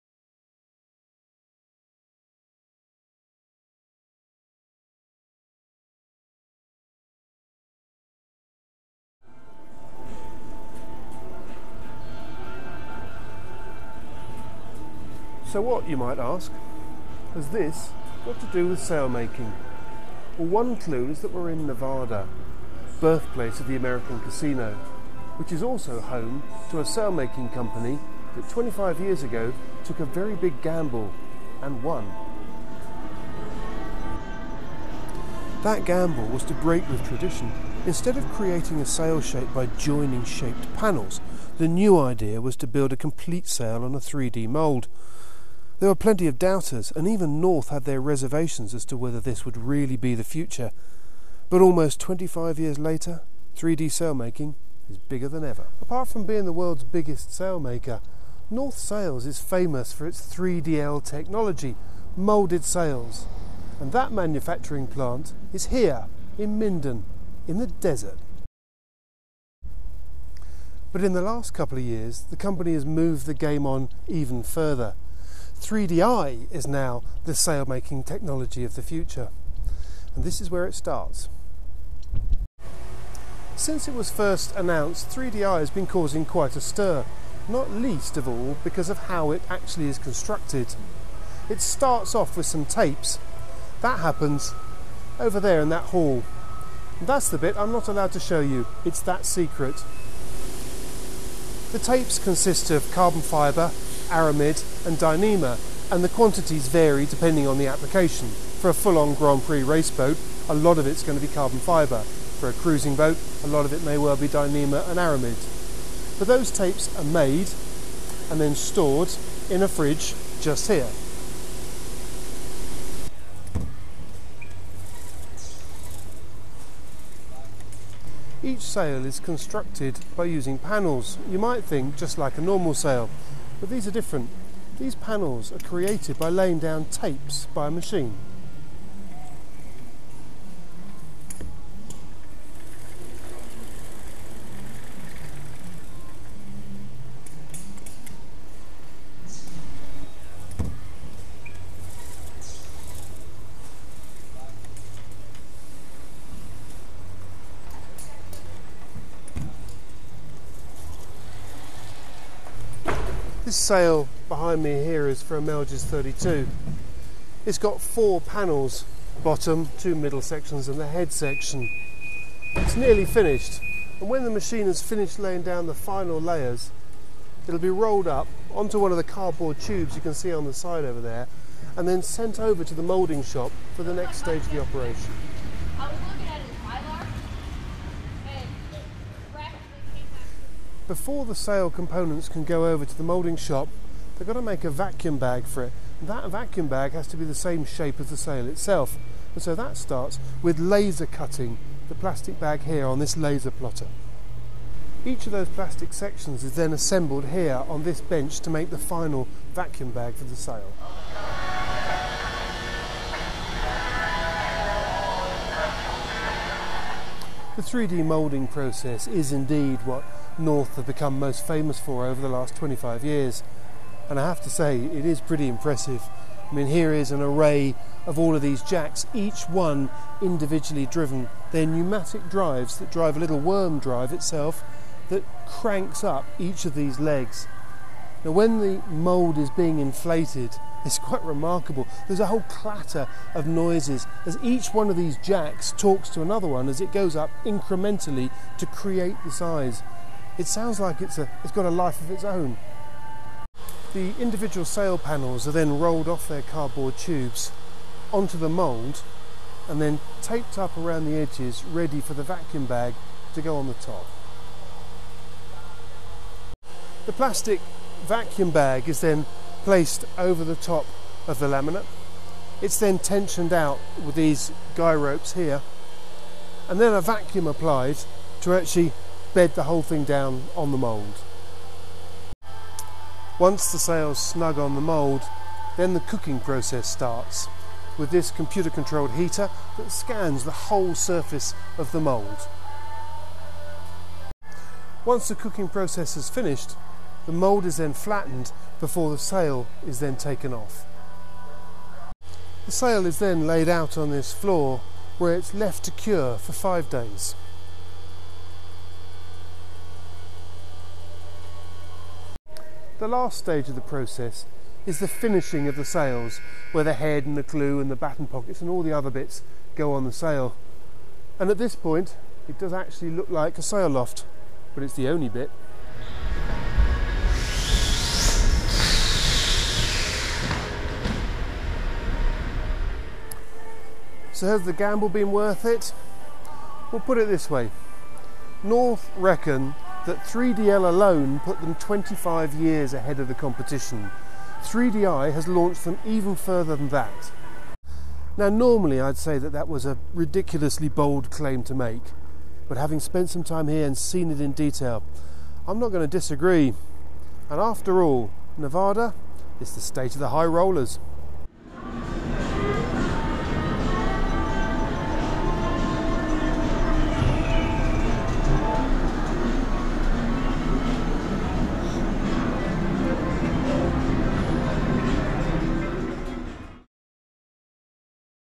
How North Sails make 3Di sails in the desert
Matthew Sheahan visits North Sails' HQ Minden, USA, to find out how the world's biggest sail maker builds 3Di sails in the desert and discovers a technology that looks set provide the next generation of sails for more than just Grand Prix boats